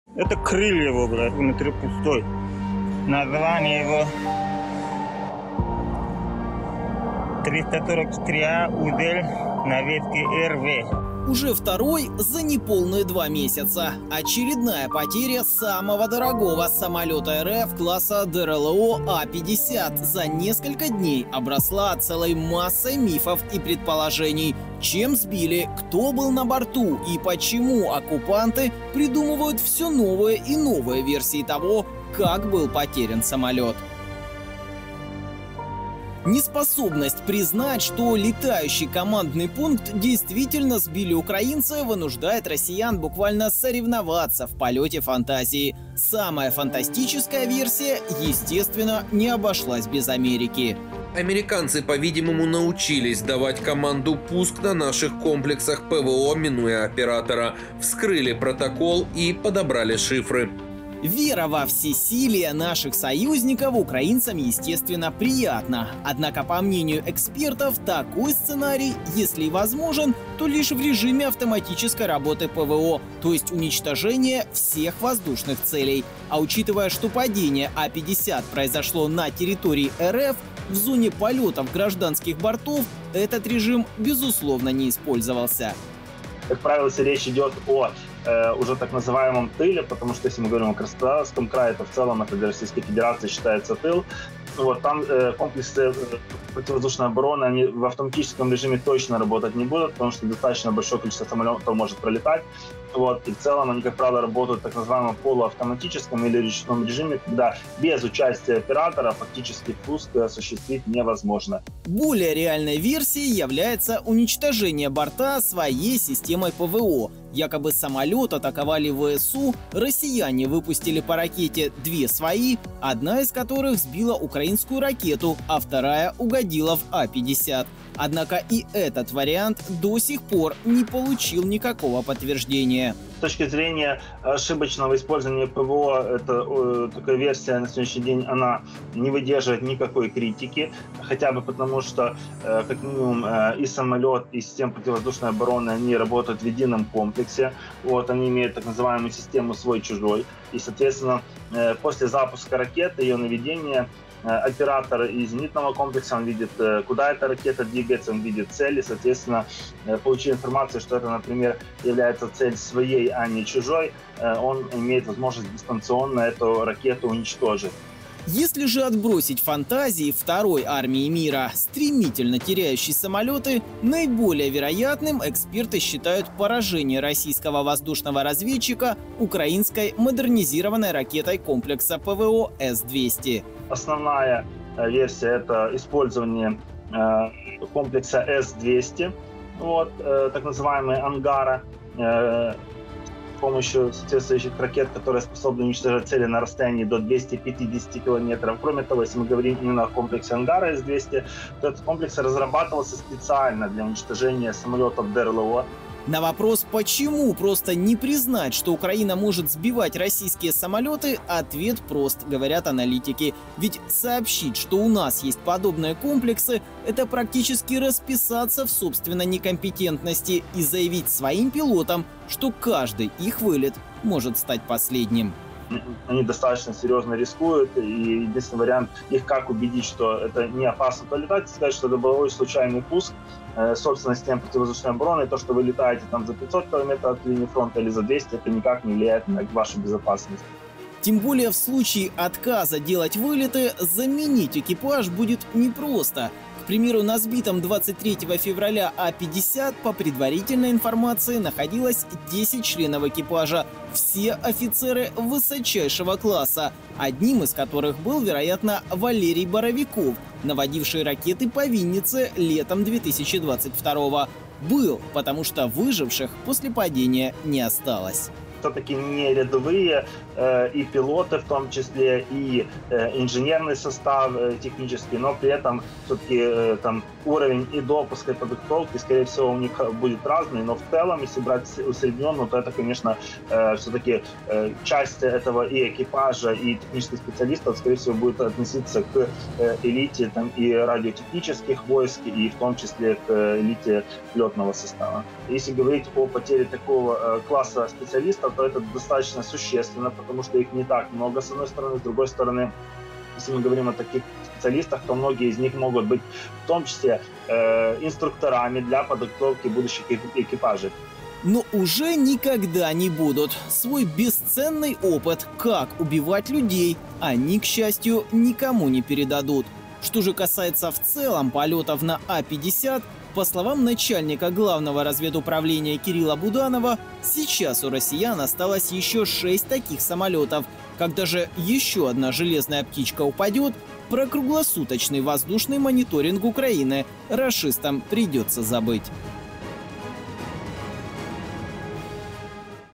💥 Как СБИЛИ А-50: всплыли детали! Был глазами россиян в небе, а стал металлолом!
Уничтожение самолета российских ВВС А-50 в небе над Краснодарским краем унесло с собой  жизни всего экипажа, а это 10 высококвалифицированных и уникальных специалистов армии оккупантов, в том числе один из них, майор Валерий Боровиков, был палачом, пустившим ракету по Виннице 14 июля 2022года, тогда погибли десятки мирных украинцев, включая детей. Расскажем о погибшей команде оккупантов и том, что в их лице потерял военно-воздушный флот РФ. А еще о том, почему российские пропагандисты упорно приписывают уничтожение самолета А-50 своей ПВО.